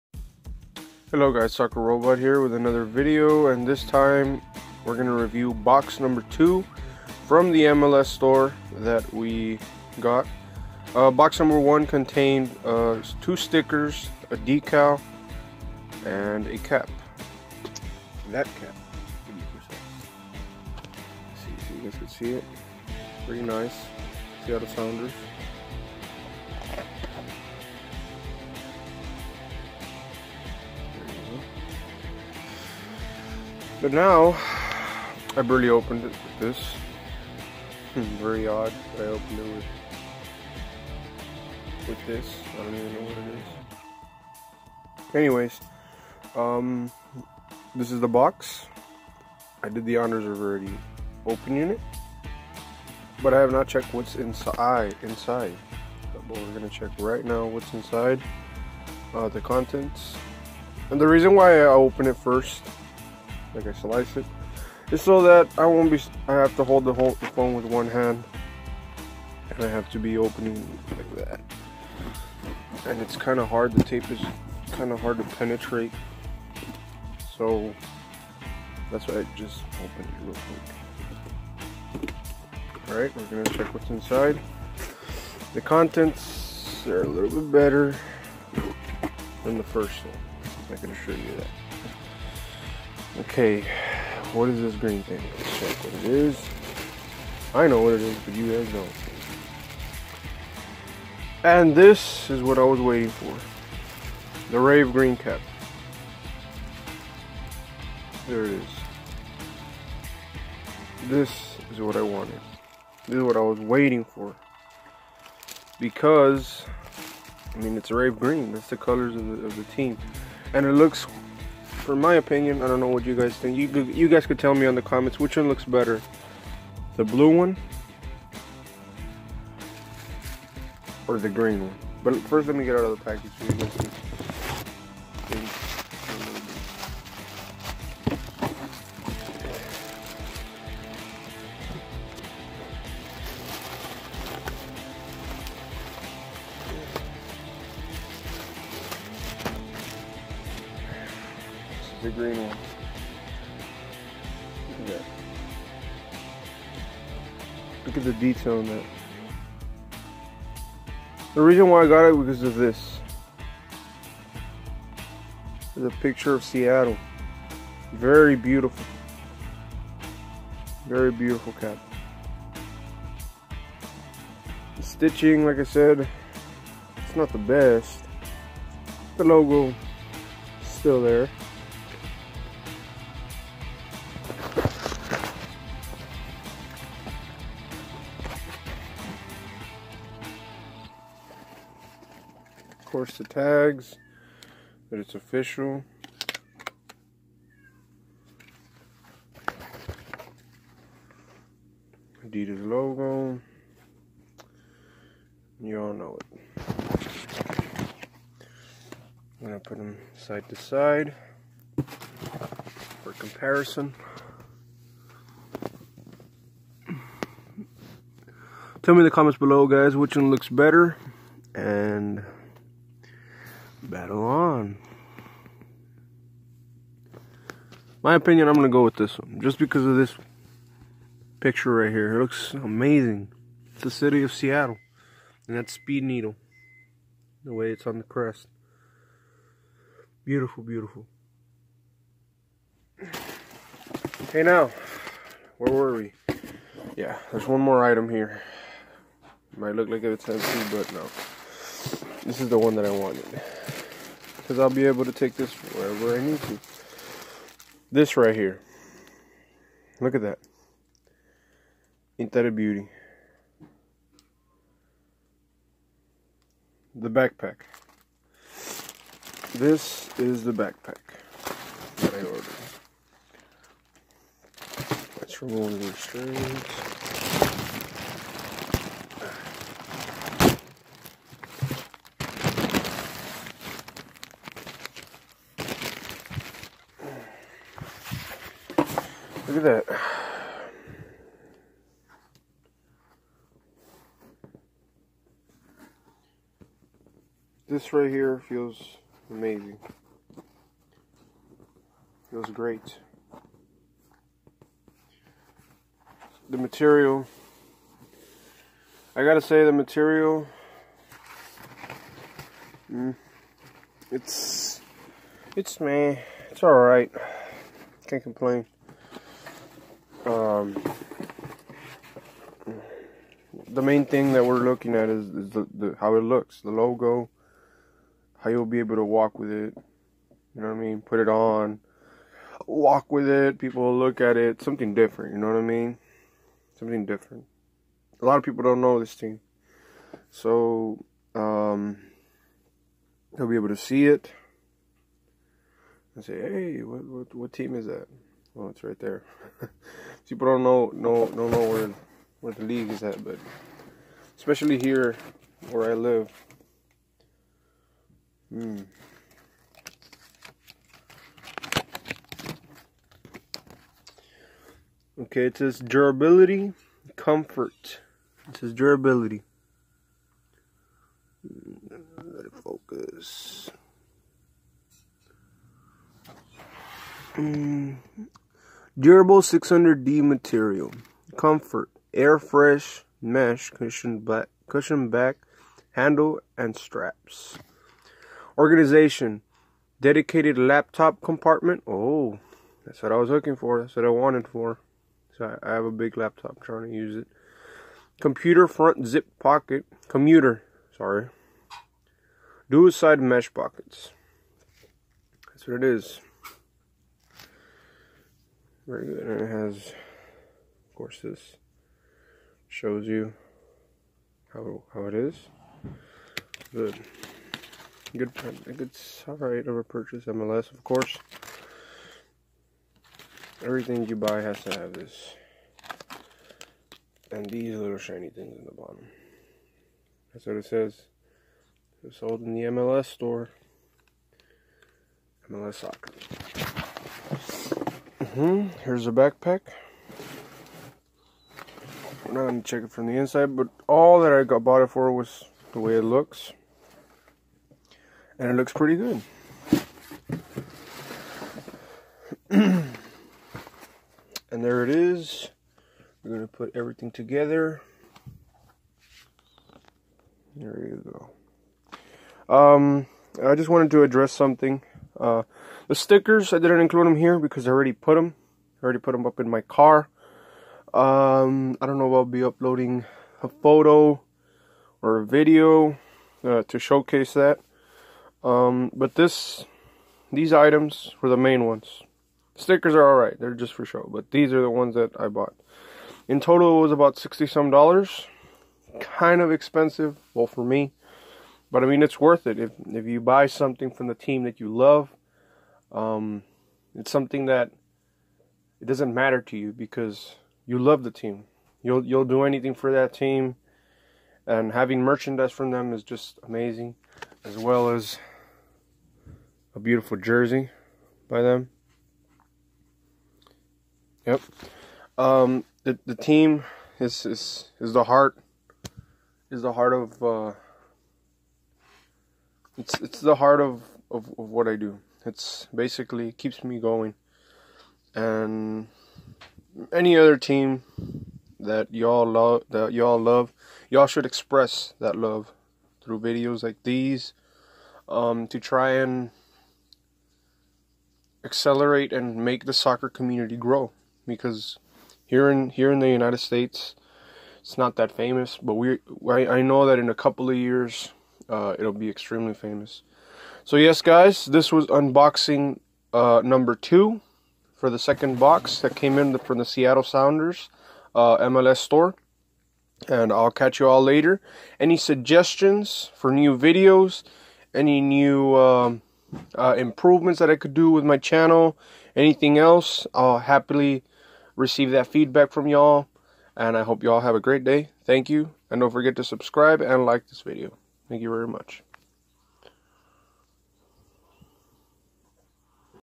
SEATTLE SOUNDERS MLS STORE MERCHANDISE BOX #2 OPENING
In this video we review box #2 That we received from the mls store.  The team featured is the Seattle Sounders FC.